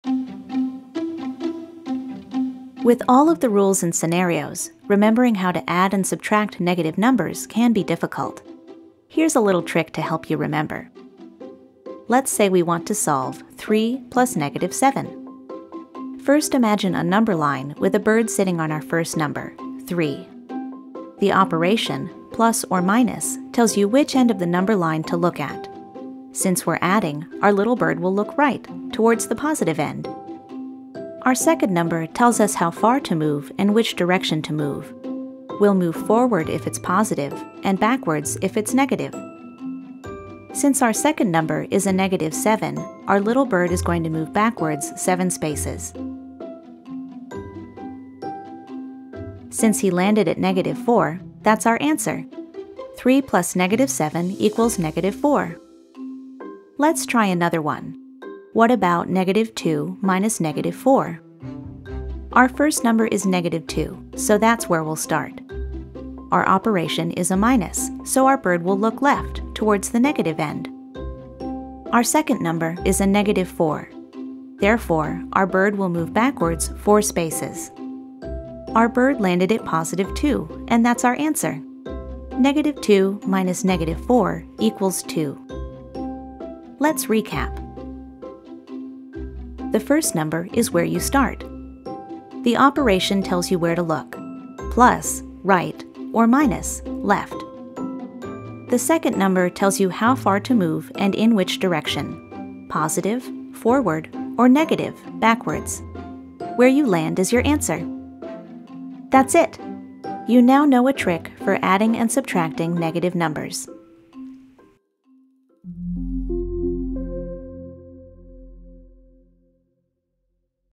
A Trick for Adding and Subtracting Negative Numbers
In this video, you’ll learn more about adding and subtracting negative numbers. for our text-based lesson.

This video includes information on:
• Adding negative numbers
• Subtracting negative numbers
• Using a number line to perform these operations